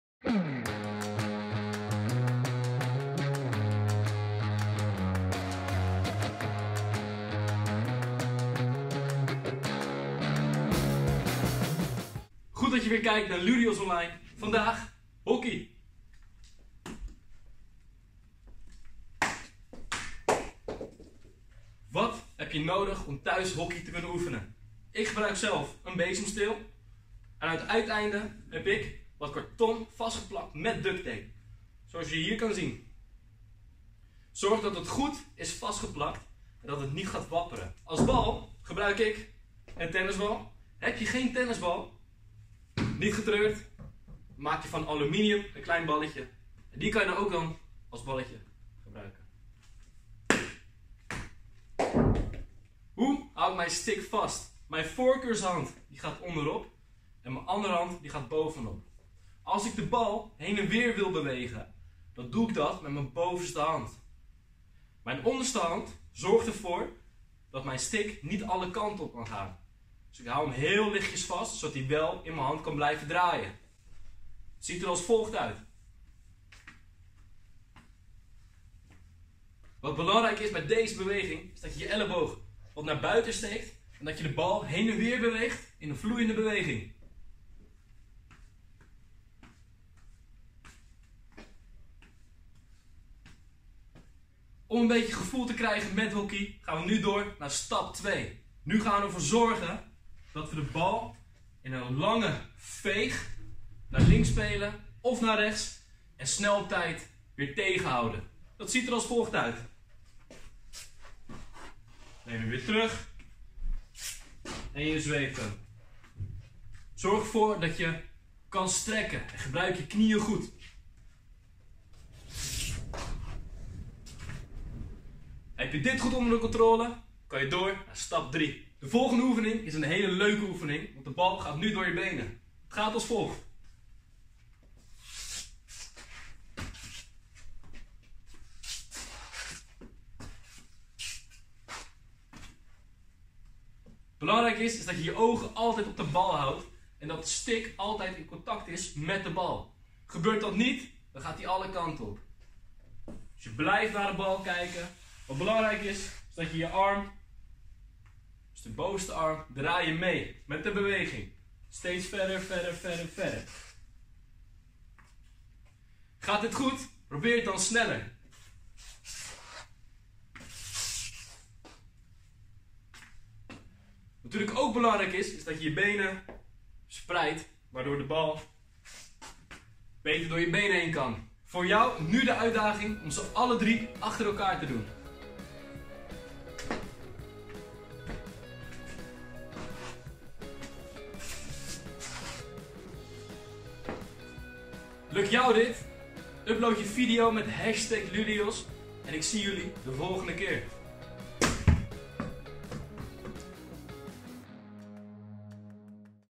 Hockey met je eigen stokkie!
Kan jij deze trucjes achter elkaar uitvoeren? Laat ons jouw hockey skills zien!

Doe een post op social media waar jij de Ludios Online activiteiten doet, voeg Ludios toe en maak kans!!